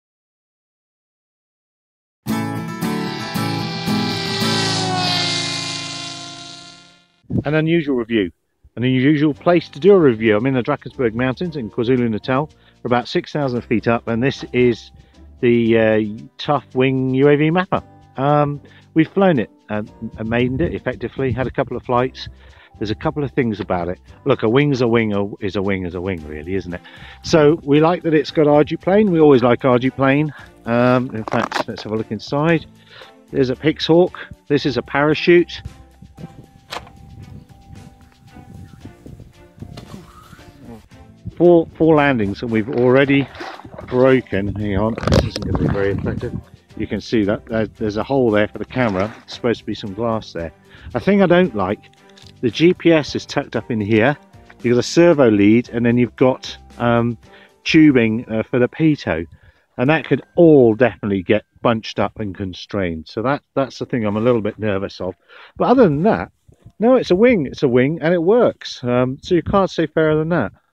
TuffWing UAV Mapper - It works!
Not much to say, a challenging day out for the TuffWing UAV Mapper, new to us and work to do. If you can use Mission Planner, you can use the UAV Mapper out of the box.

Small niggles but it matches much more expensive systems for performance.